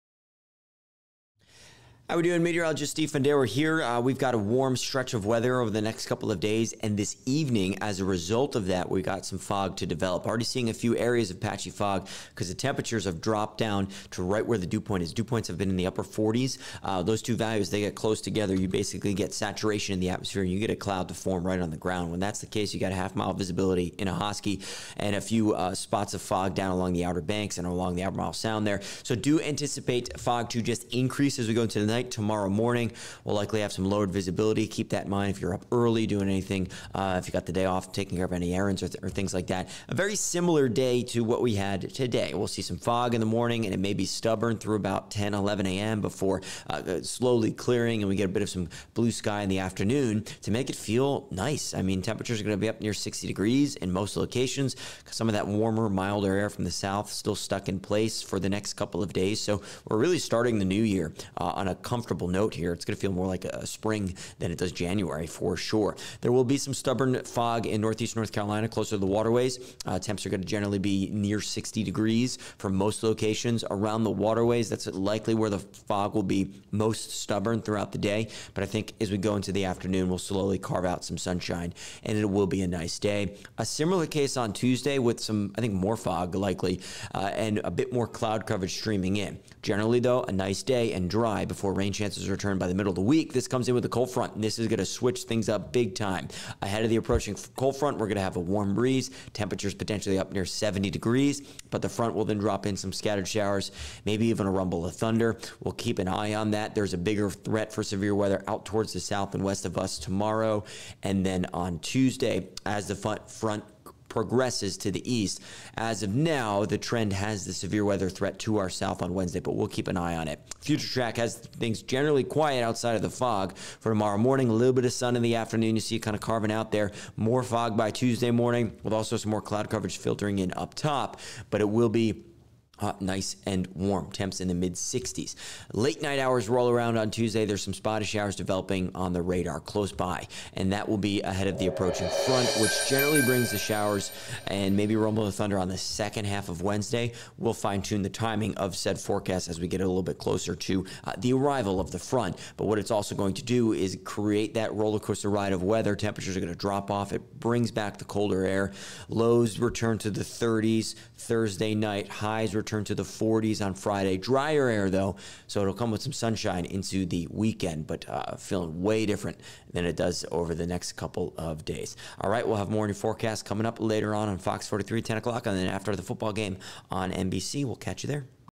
Foggy & warm! | January 1, 2023
As the warm air holds, we'll likely see a few rounds of fog for the next few days.